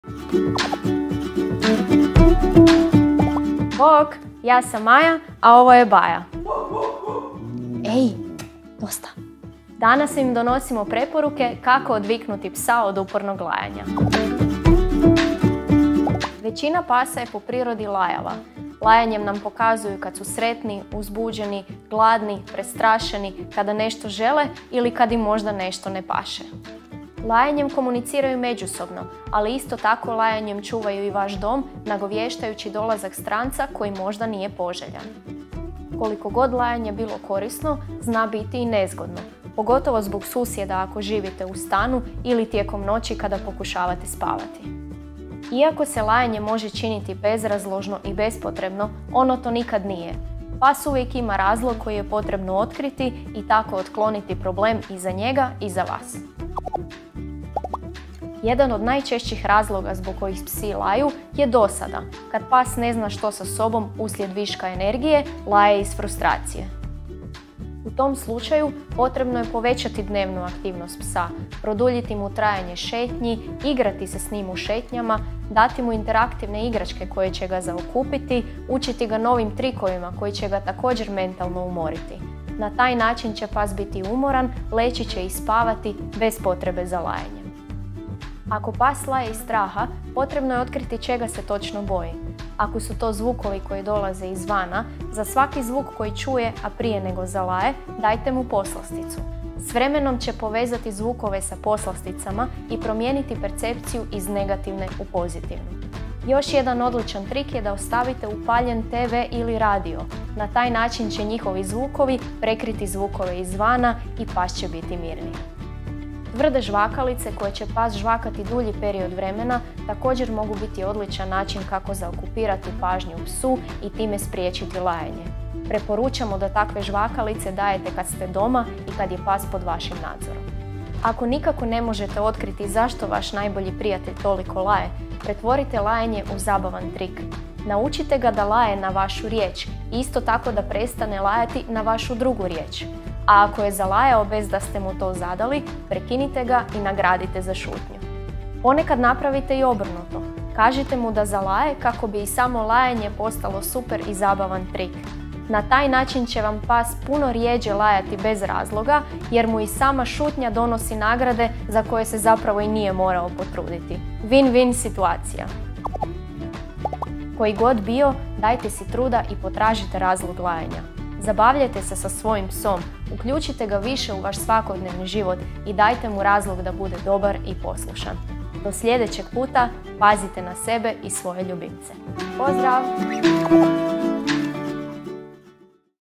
Što kada pas stalno laje? - VLOG veterinarke Maje Črnjević
[TVOJ PAS NEPRESTANO LAJE?] 🐶 U današnjem VLOGU naša ti veterinarka Maja otkriva iz kojih sve razloga tvoj ljubimac može lajati te kako mu ti možeš pomoći ako je u pitanju strah ili višak energije.

Ako već nisi, pretplati se na naš 🎬 YouTube kanal i svakog ponedjeljka otkrivaj novosti iz svijeta kućnih ljubimaca!